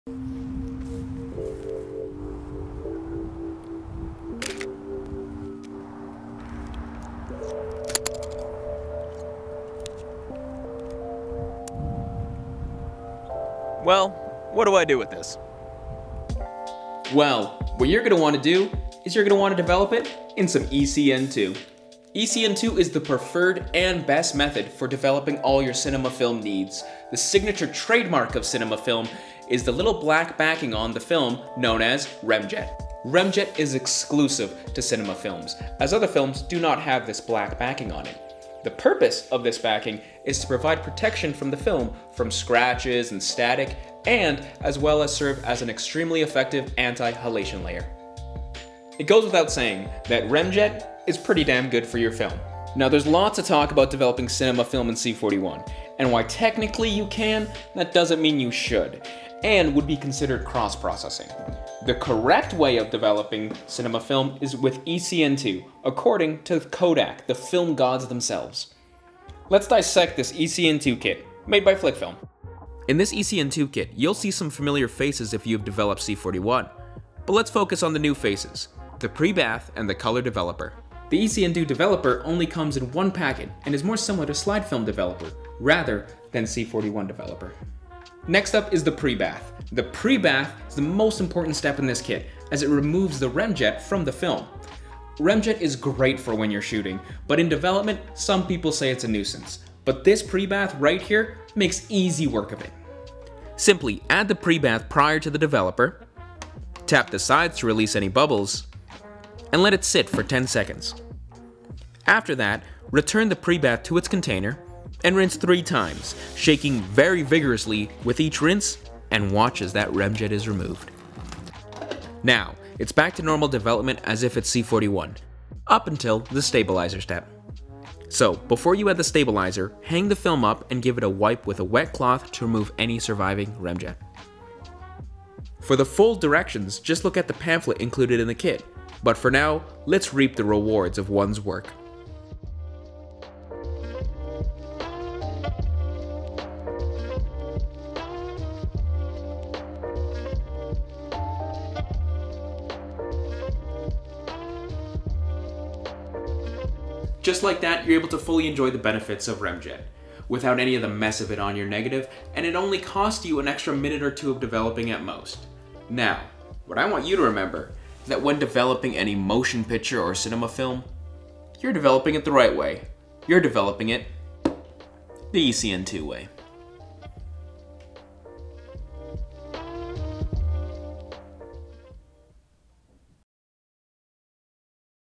Developing ECN-2 at home
ECN-2 is the preferred method of developing your motion picture/cinema film. In this video we show you the basics and the purpose of this ECN-2 kit made by FLIC FILM